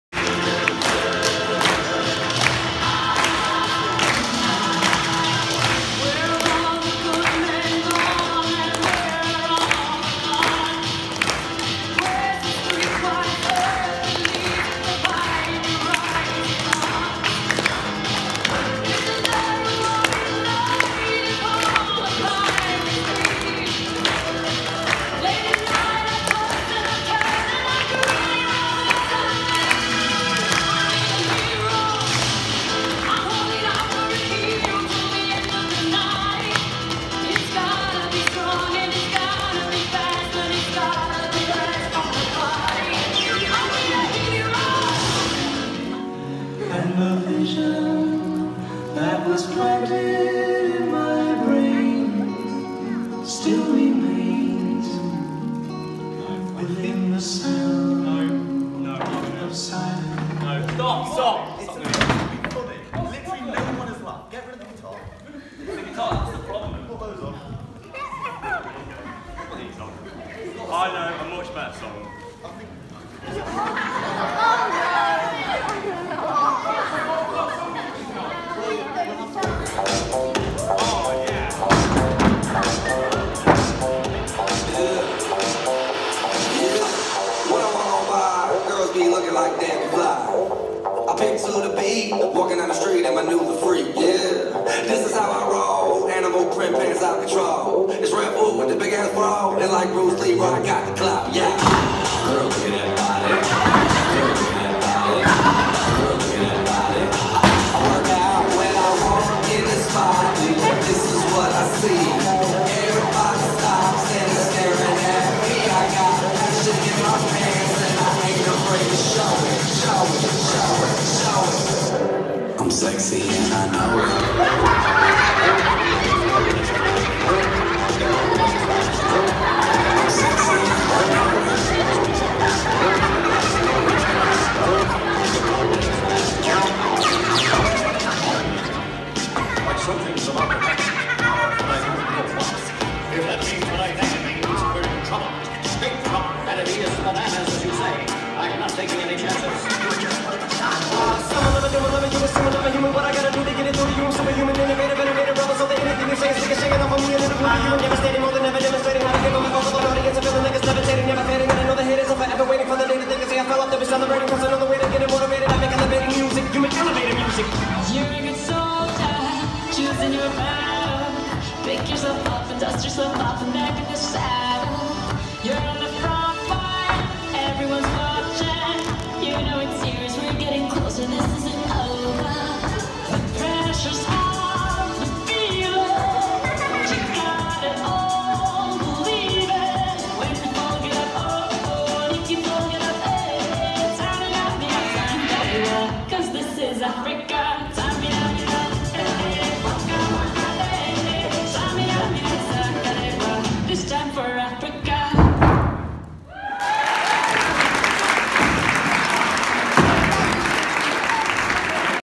Lip Sync Battle 2017
Teachers vs Pupils battle it out in the 2017 Lip Sync Battle - all to raise money for our Tanzania Project 2018